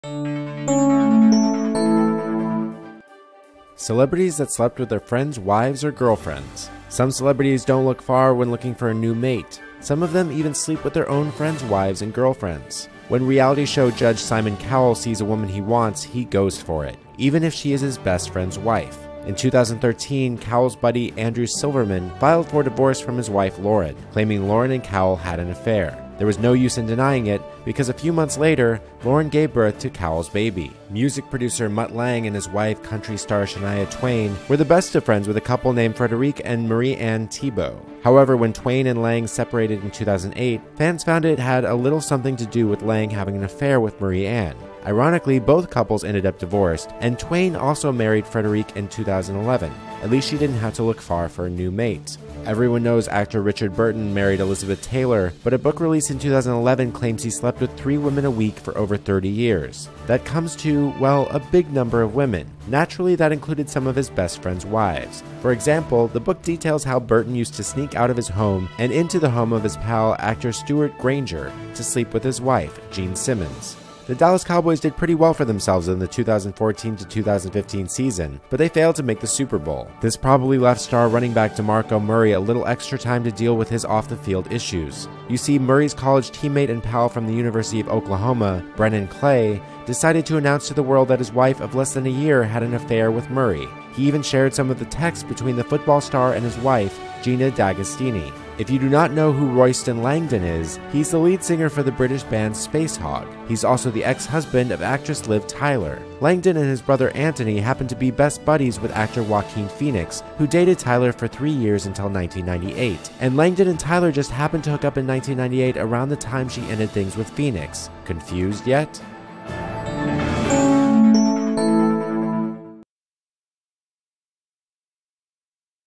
Celebrities That Slept With Their Friend's Wives or GFs
Oh no, they didn't! Some celebrities don't look far when looking for a new mate. Some of them even sleep with their own friends' wives and girlfriends.